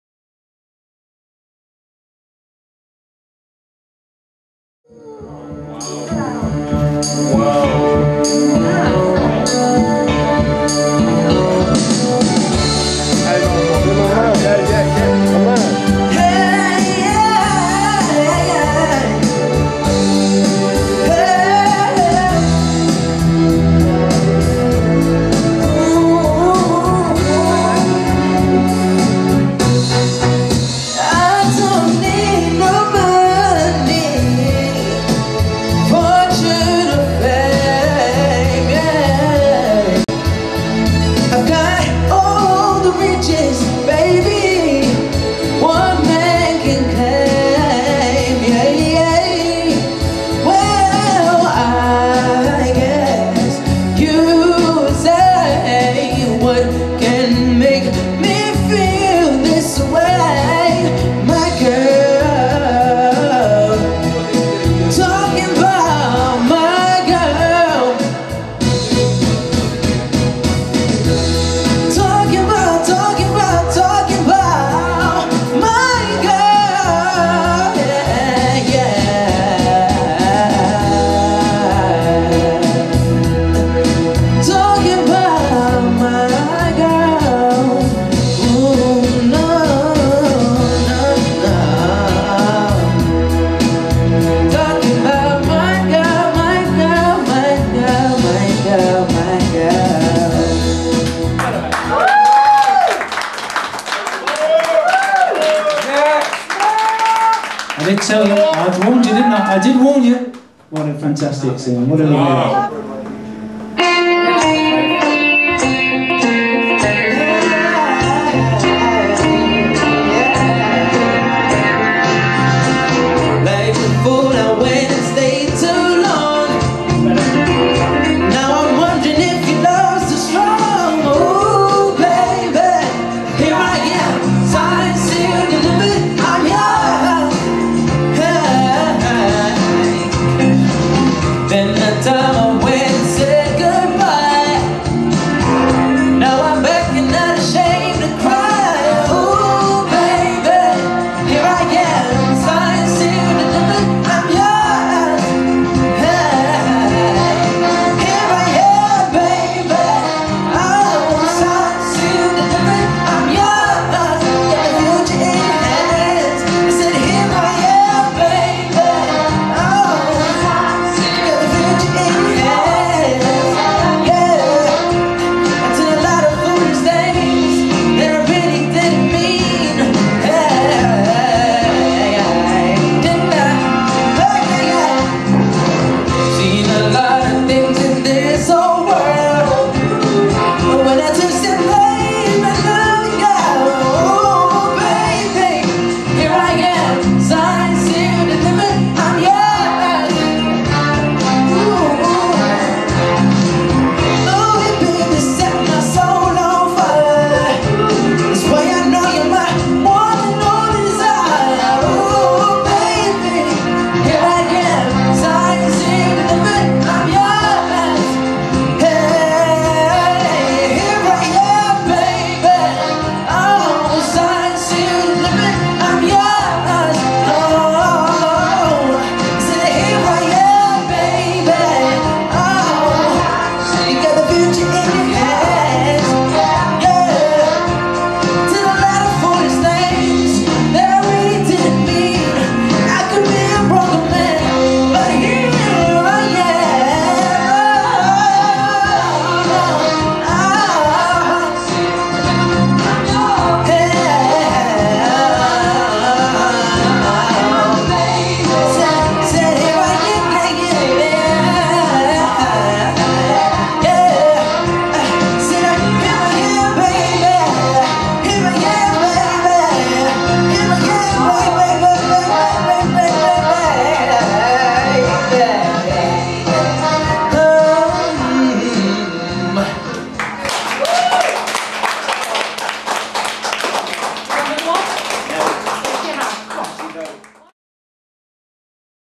Kieran Alleyne at Sansomes Bar - Dec 2nd 2012
Kieran Alleyne at Sansomes Bar, Leicester.
Open mic night. Sunday 2nd December 2012.
Filmed by IgnitionOpenMic.